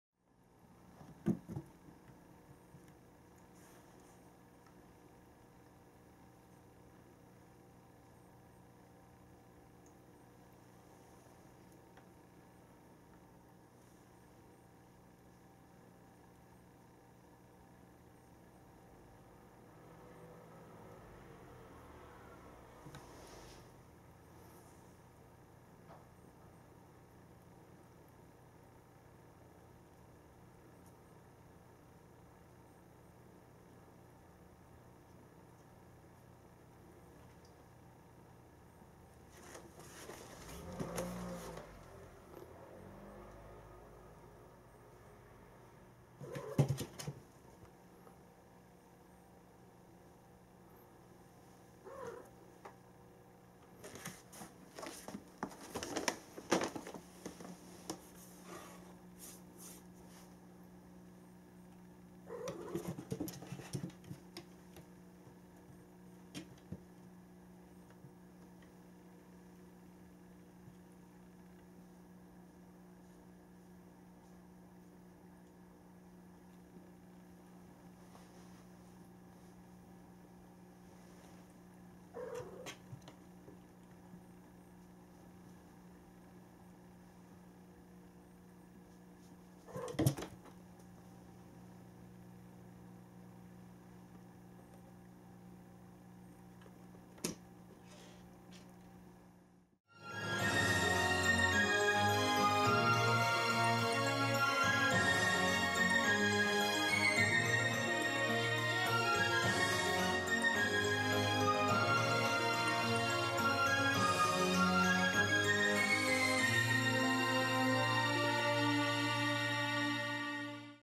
影が気になる/Shadow worried about -ノルウェージャンフォレストキャット/Norwegian Forest Cat-
他の動画を撮ってたら影に気づいてしまい、夢中になっちゃいました。
影は捕まえられないよ・・

It will be aware of the shadow If I had taken the other videos, it has become crazy.
It is not caught shadow ...